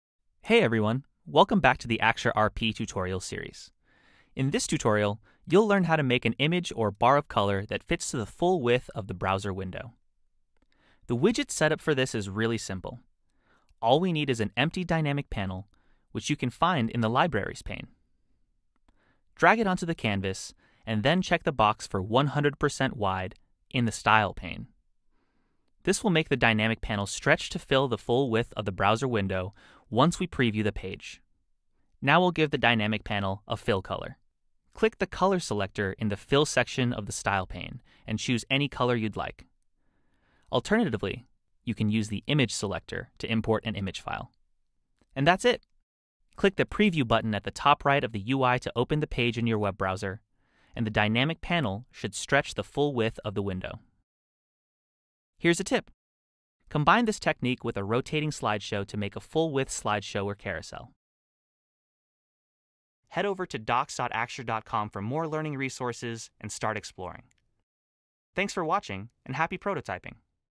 Full Width Images and Banners in Axure RP
In this Axure RP tutorial, you'll learn how to make an image or bar of color that fits to the full width of the browser window.

To read a written version of this tutorial and download the completed prototype file, head over